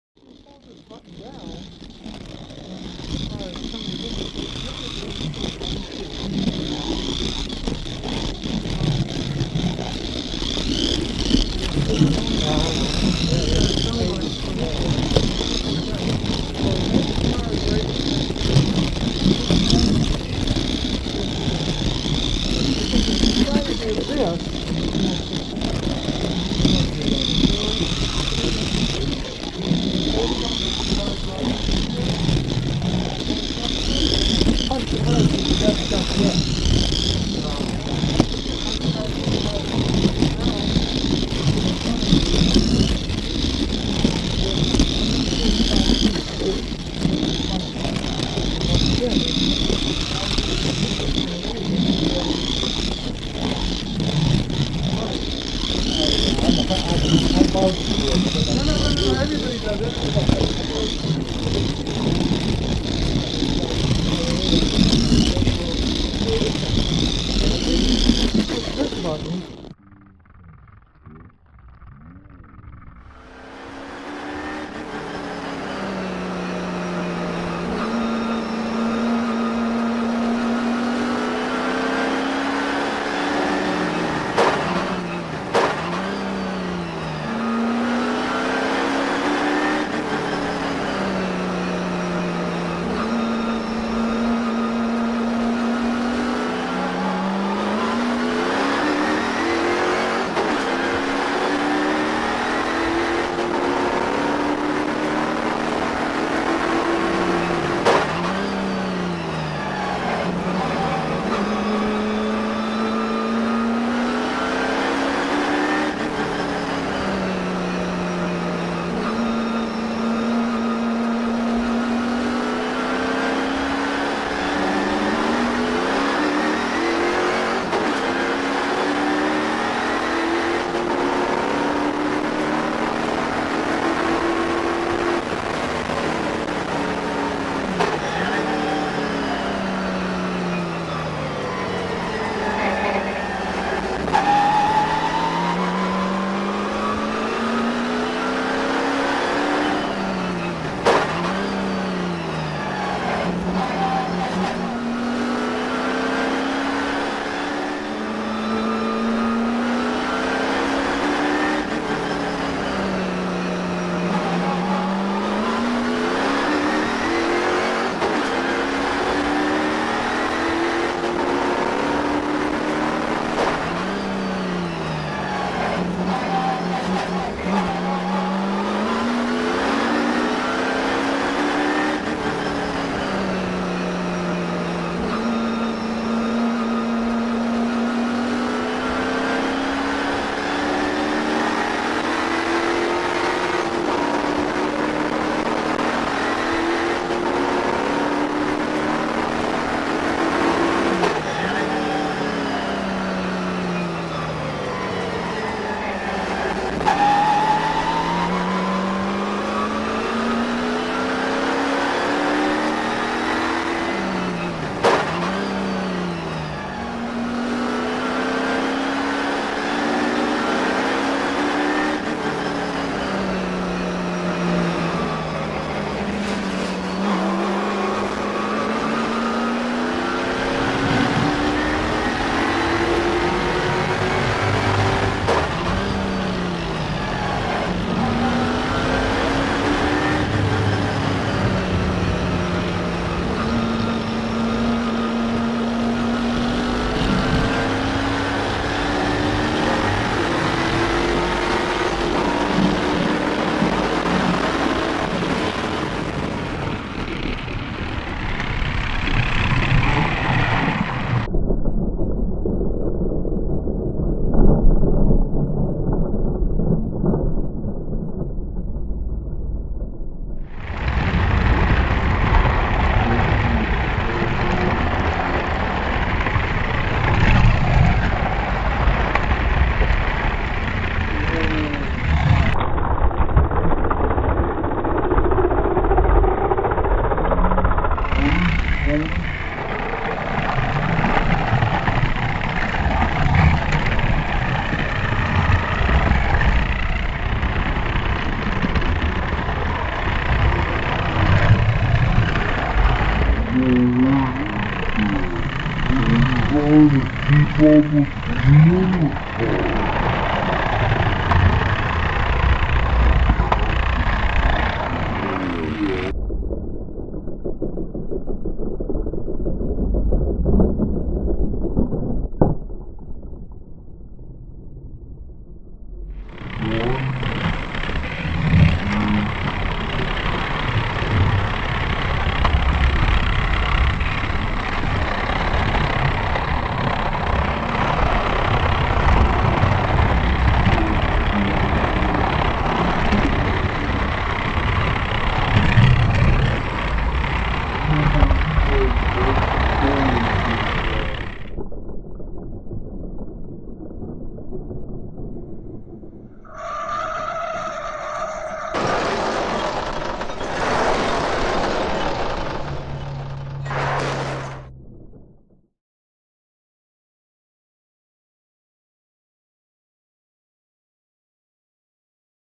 Slot car RACING at Northline Raceway Car view! - Full Version, As requested *
This is the full video as requested - enjoy :)

This is an old camera on car clip from 2014 - it is somewhat blurry. We will be adding new racing, on car videos and more how-to soon. in the meantime, enjoy these old clips from Jimmy's video vault.

*This was not filmed by LK, Credit: Steve Kouzoujion
Thank you Steve

Detroit Free Press article about Jimmy's wonderful track!